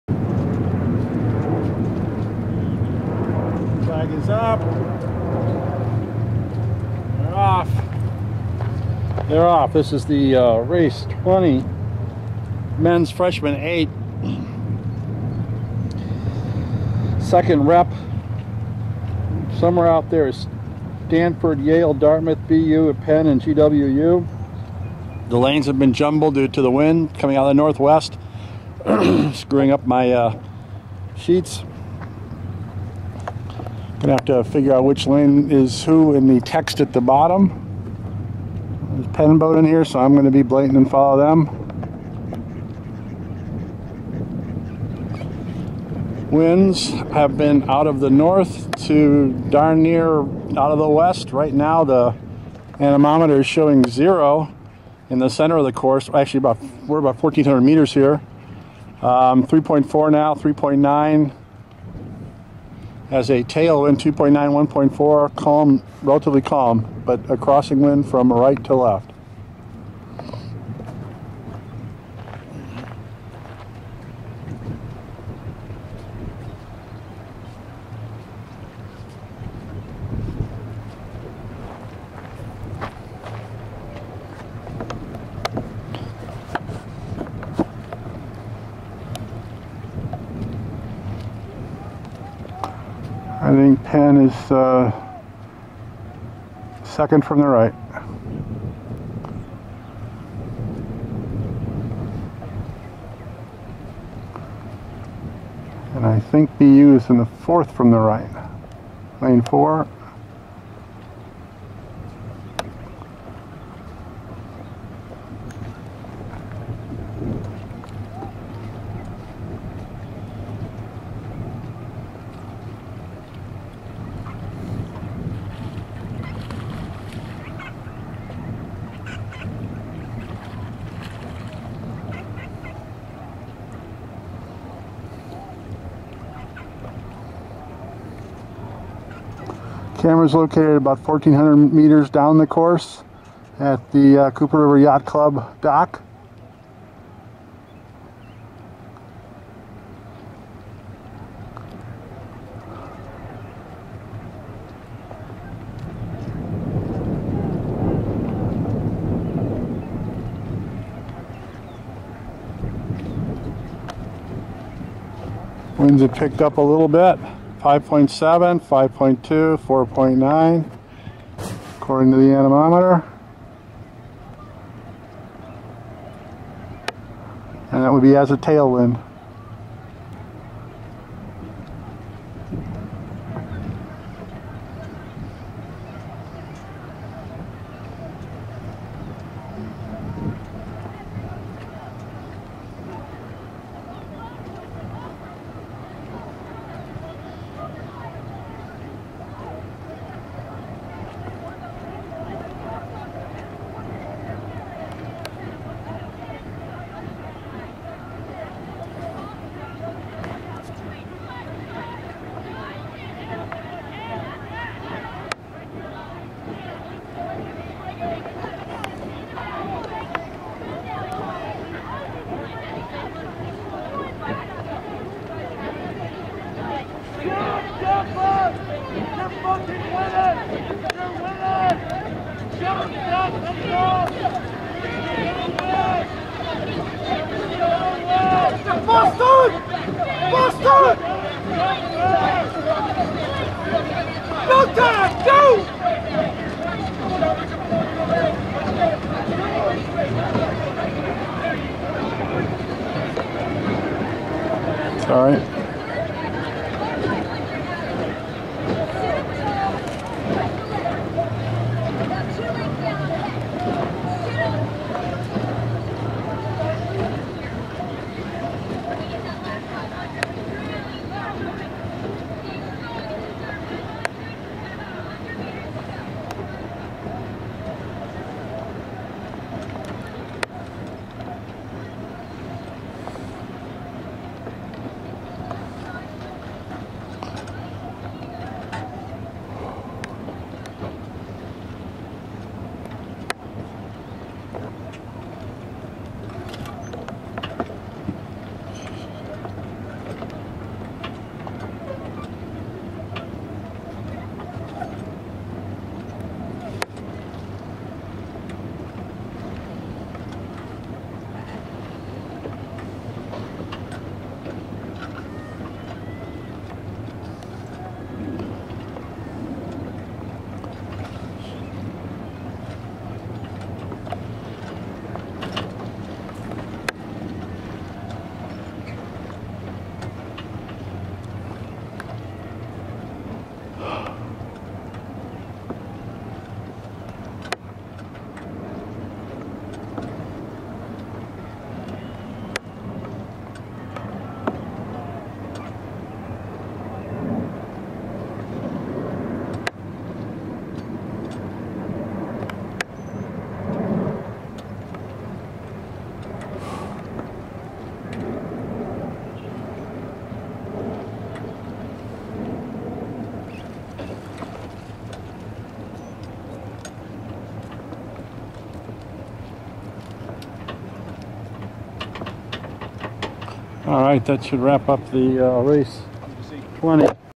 2012 IRAs d1 20 MF8 Rep2 Boston U Yale Dartmouth Stanford Penn GWU Rowing Crew
Lanes 1 to 6, Far to Near: Stanford Penn Yale Boston U Dartmouth GWU. Watch it in hi-def.
Finish Order: Boston U 5:51.175, Yale 5:52.278, Dartmouth 5:53.874, Stanford5:54.229, Penn 6:00.128, GWU 6:21.386.
2012 IRAs, Day 1, Race 20, Mens Freshmen 8 Repechages #2: First 3 to Semis, Second 3 to Thirds.
May 31, 2012, 15:15 start, Cooper River, Cherry Hill, NJ.
Camera location on the Cooper River Yacht Club dock, about 1,400 meters down the course, on the south side of the river. If you look closely, silhouetted on that brown roof, you can see the starting judge wave the flag.
Weather: Mid 80s F. Winds fluctuating from West to North, averaging out of the Northwest. Tail winds measured by a directional anemometer (along the course) at the camera site during this race ranged from 0 mph to 5.7 mph for this race. Call it an average of 3.4 mph.
Thanks to the Cooper River Yacht Club for allowing this video to be shot from their dock.
Cooper River conditions at 15:15, Stage 1.38ft, Flow 0.0118kcfs, as measured at Haddonfield, about 4,000m upstream of the finish line at the dam.